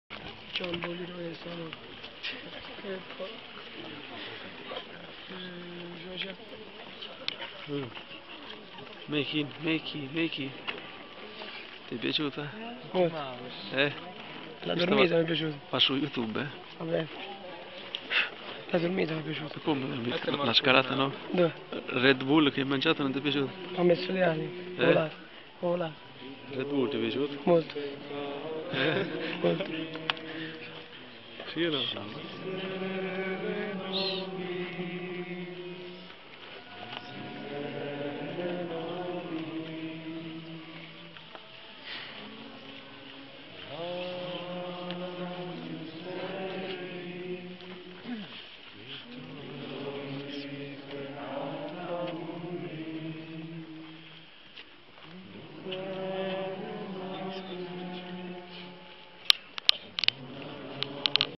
monte krizevac medjugorje
Santa Messa sul Monte Krizevac a Medjugorje. ore 5.00 del mattino.